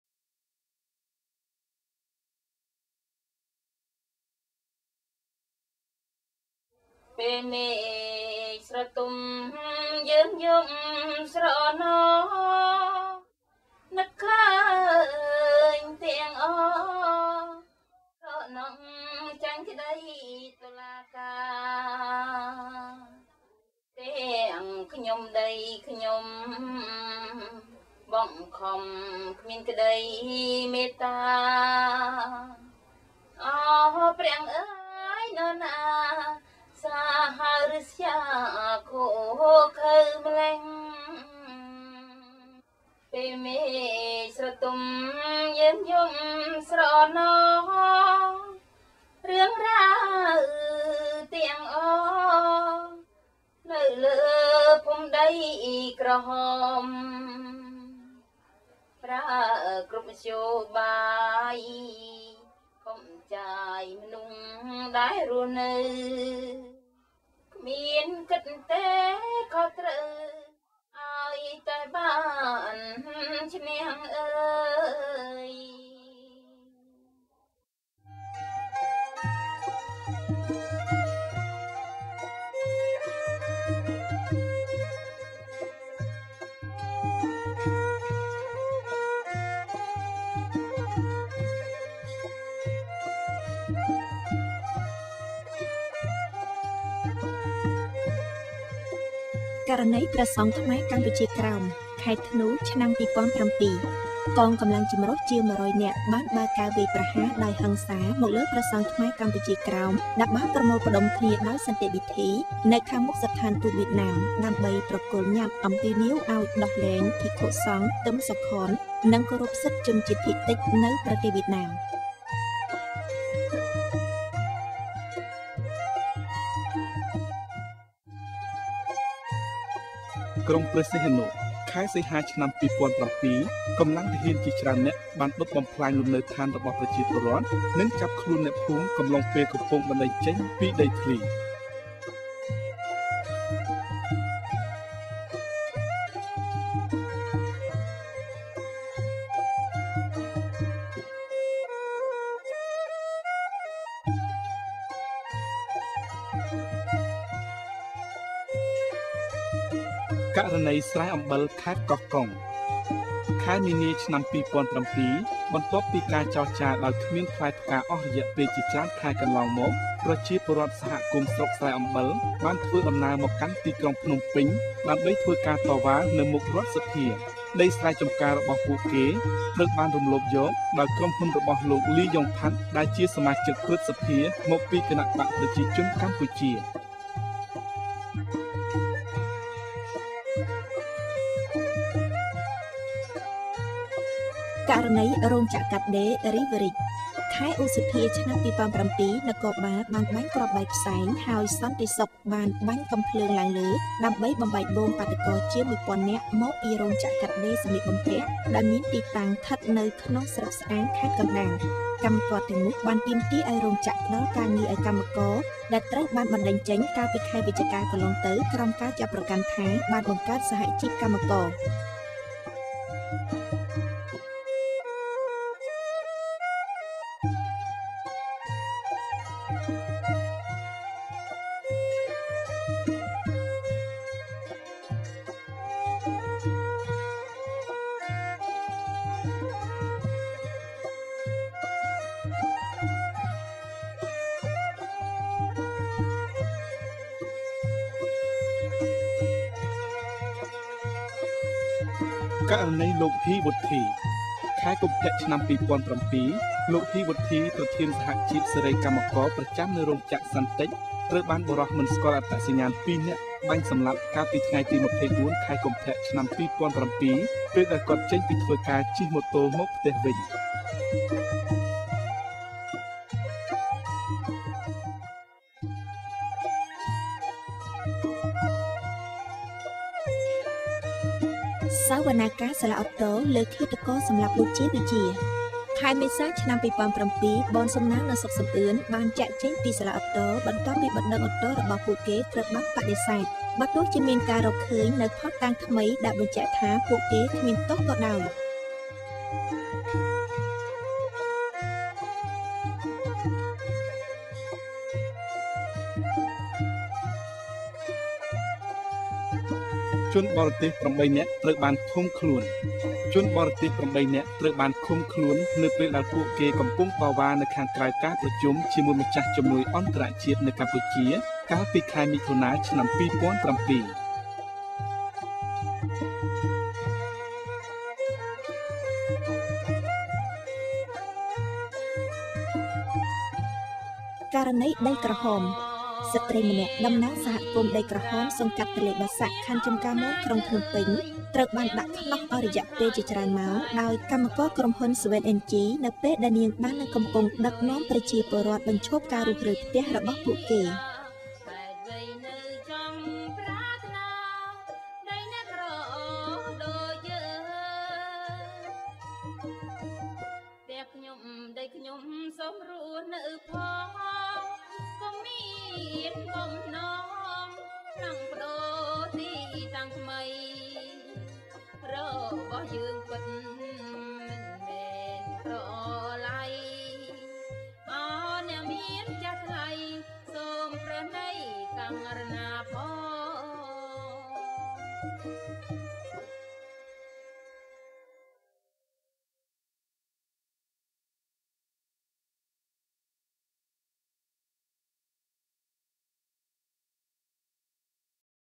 Attacks and Threats against Human Rights Defenders 2007
LICADHO's video on Attacks & Threats Against Human Rights Defenders in Cambodia 2007.